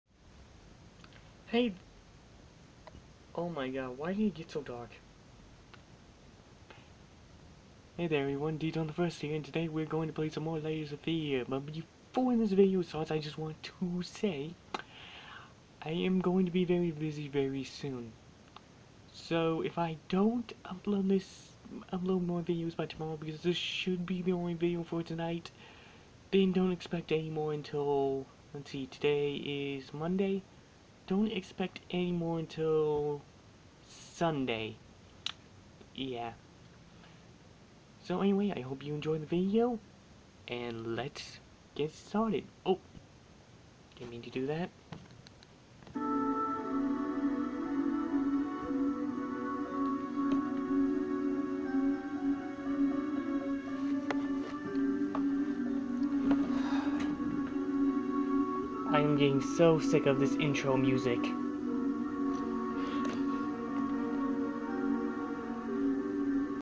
before i do layers of fear
its just a heads up before the real video uploads. Edited i mite be able to make and upload a video sooner then i thought but im not sure yet we will see.